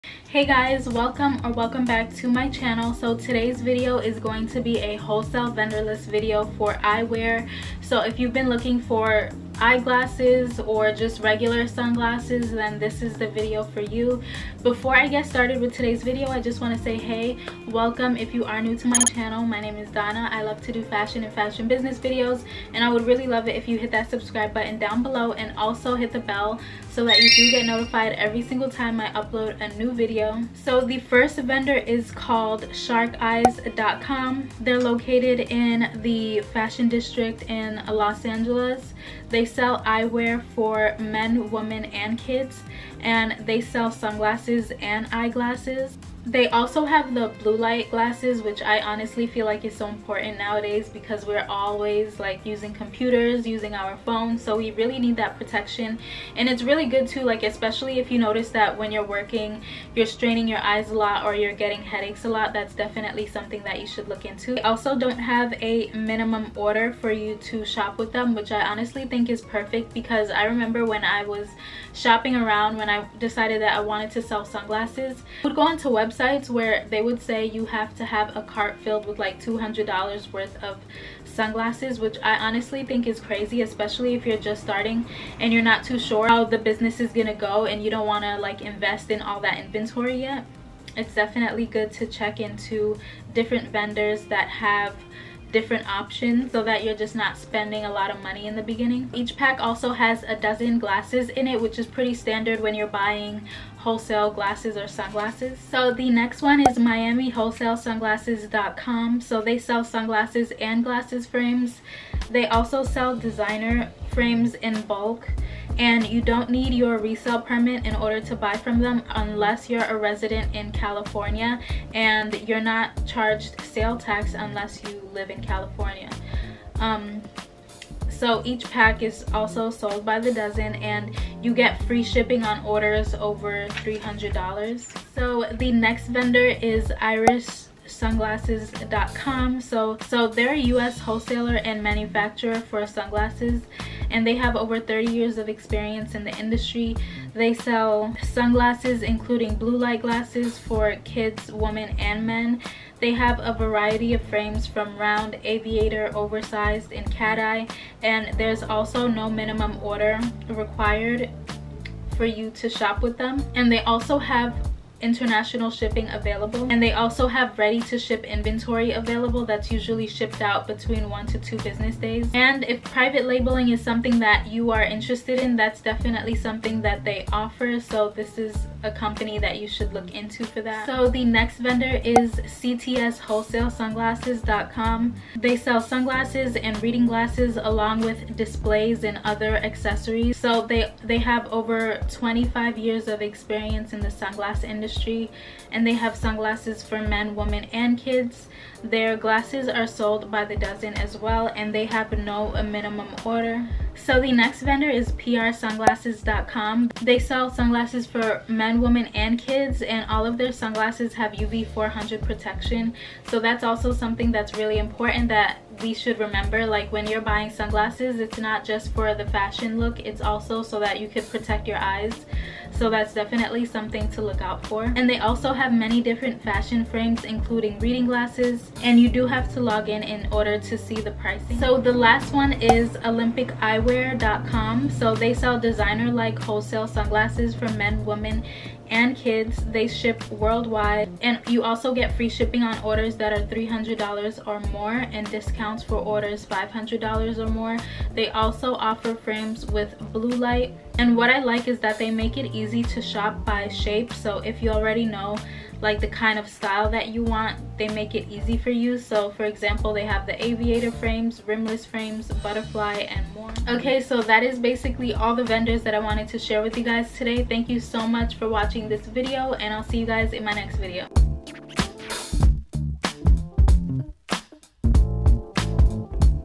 6 WHOLESALE EYEWEAR SUPPLIERS FOR YOUR ONLINE BUSINESS including private labeling + blue light
6 WHOLESALE EYEWEAR SUPPLIERS FOR YOUR ONLINE BUSINESS including private labeling + blue light

Timestamps:

Intro 0:00- 0:31
1st vendor 0:31- 1:46
2nd vendor 1:46- 2:16
3rd vendor 2:16- 3:00
4th vendor 3:00- 3:24
5th vendor 3:24- 3:51
6th vendor 3:51- 4:23
outro 4:23- 4:37

HELPFUL VIDEOS

Romans 15:13 ❤️